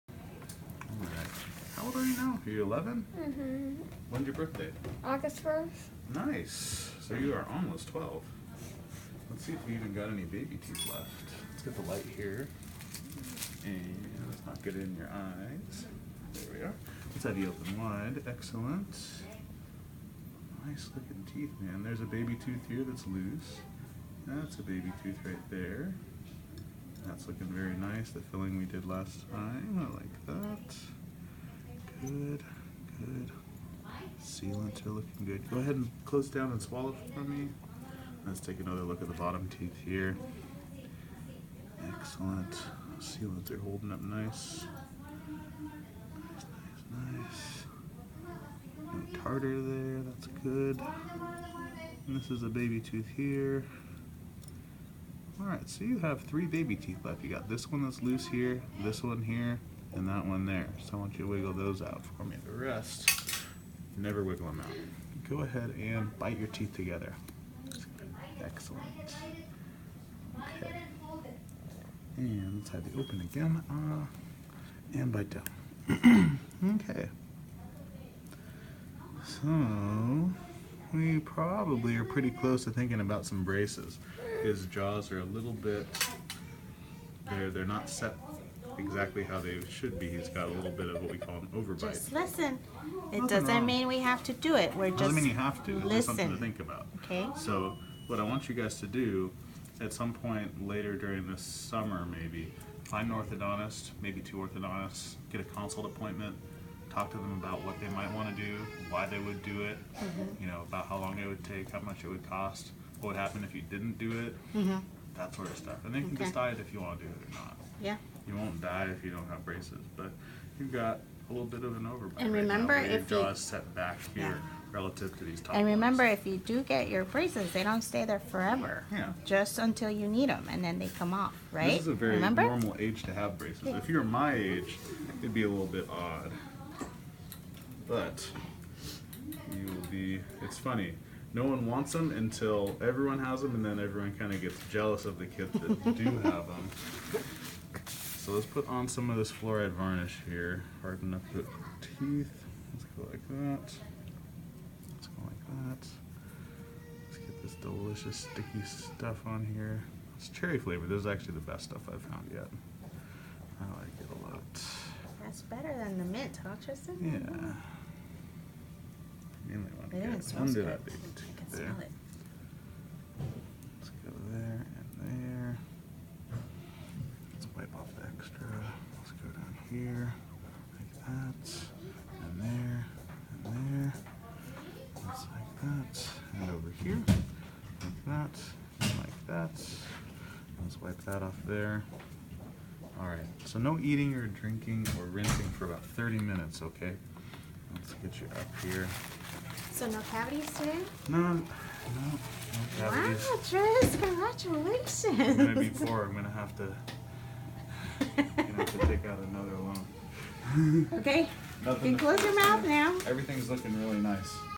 Dental Visit
If you have autism, going to the dentist can be scary. But here you can see that there's not much to it. As long as you take care of your teeth, a dental check up is a breeze.